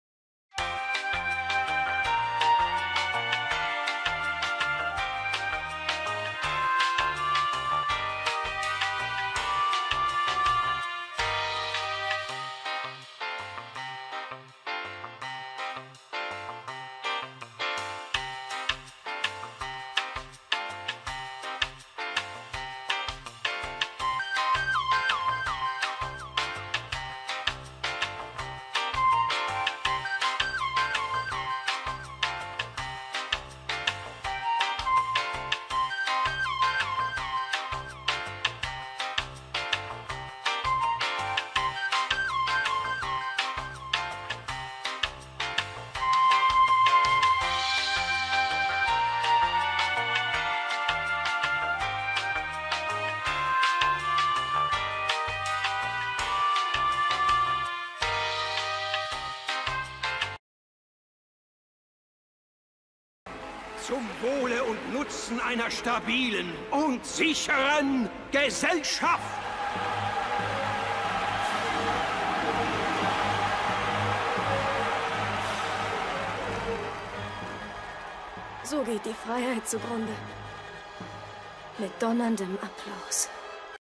So sah (gefühlt) die Abstimmung über die neue Urheberrechtsreform aus.
Dieses Video ist für alle die gegen Artiel 11,12 und 13 der Urheberrechtsrefom gekämpft bzw. demonstriert haben. Ich leide mit euch. Nur noch 2 Jahre bis Doomsday.